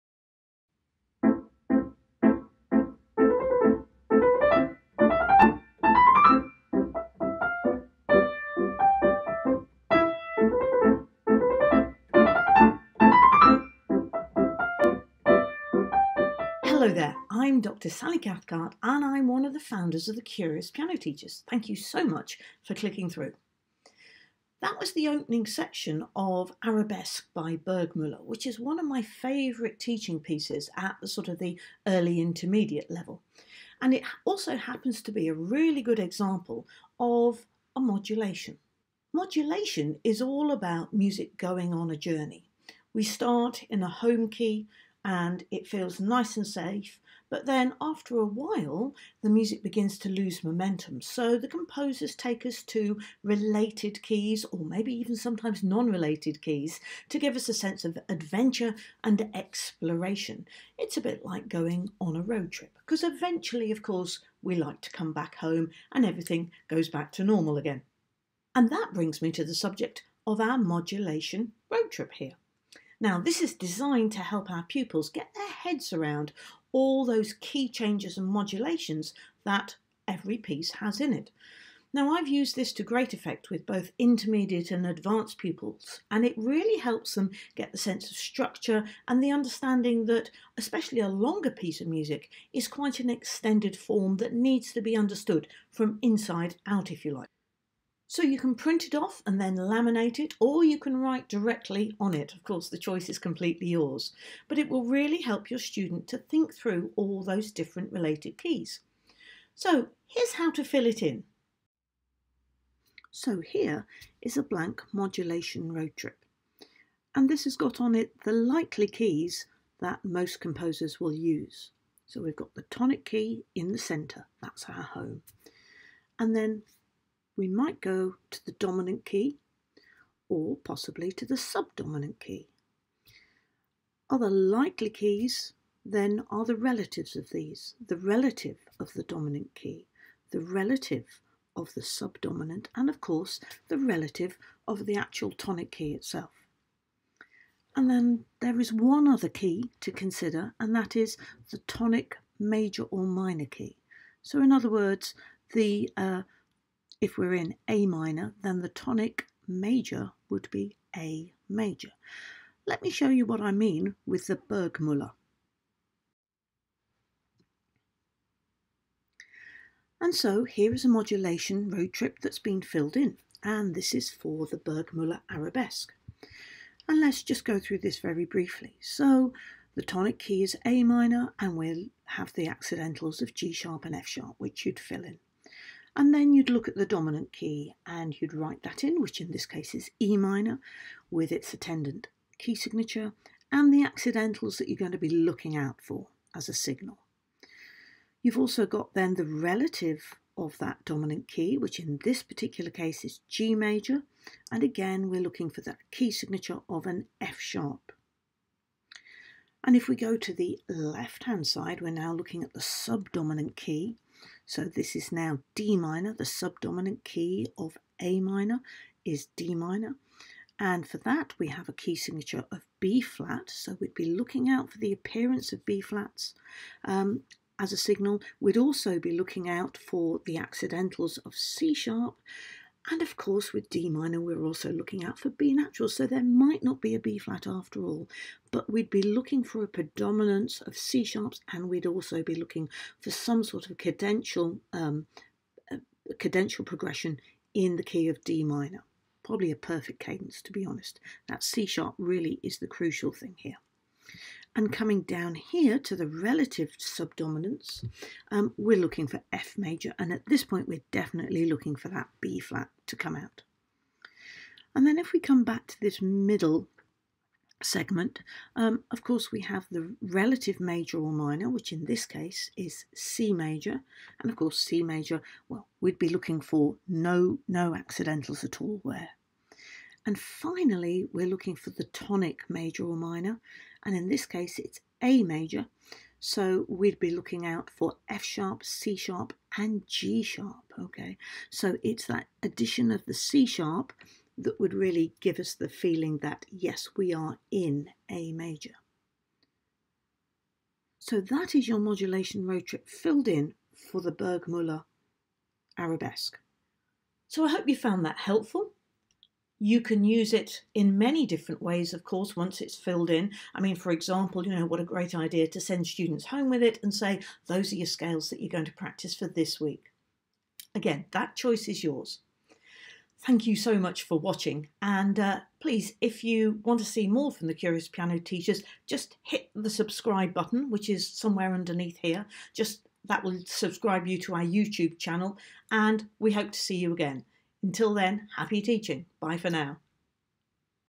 How to use your Modulation Roadtrip
Modulation is all about the music going on a journey, on a road trip if you like. Use this resource to help your students analyse key changes in the music they play.